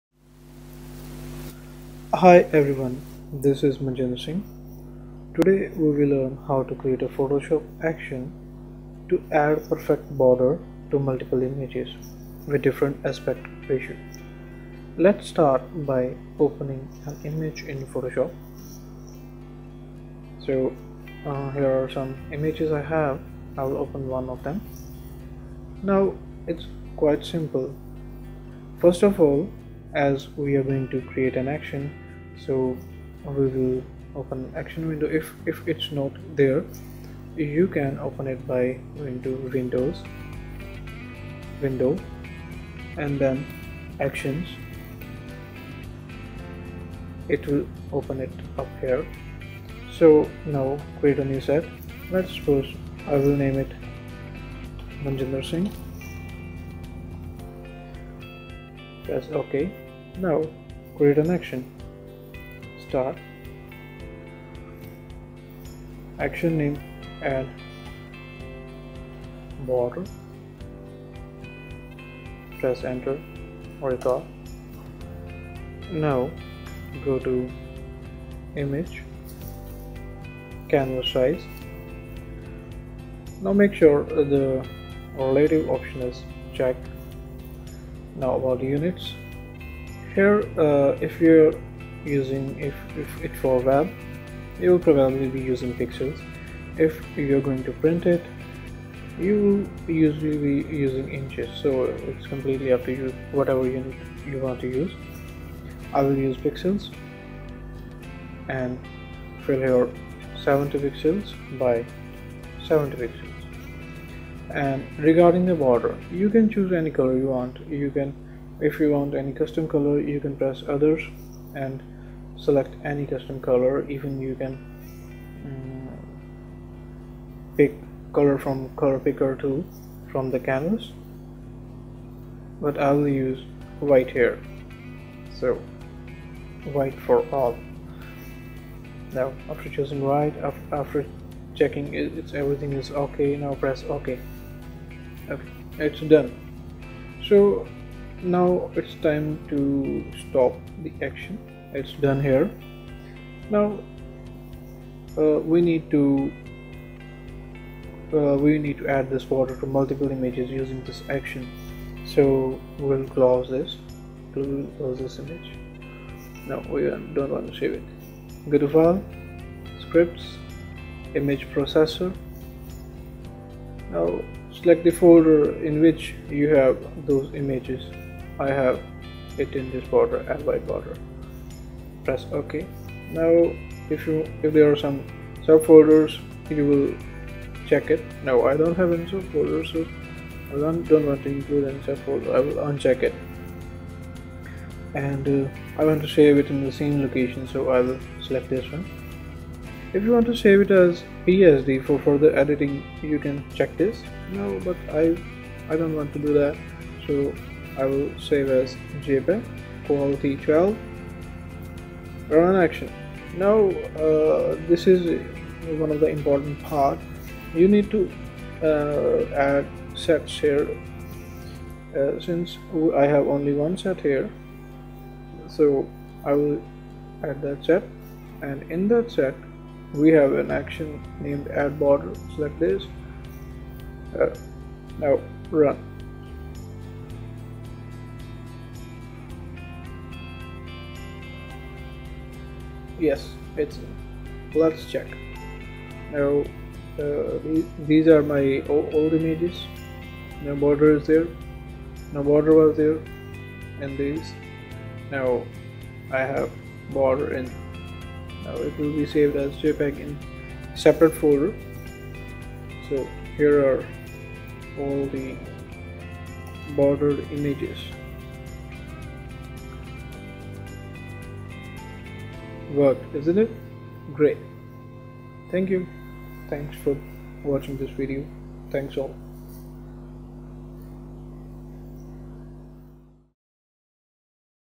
Photoshop action for adding border to multiple images with different aspect ratio
Tutorial -How to create photoshop action for adding border to multiple images with different aspect ratio.

Best Regards,
Manjinder Singh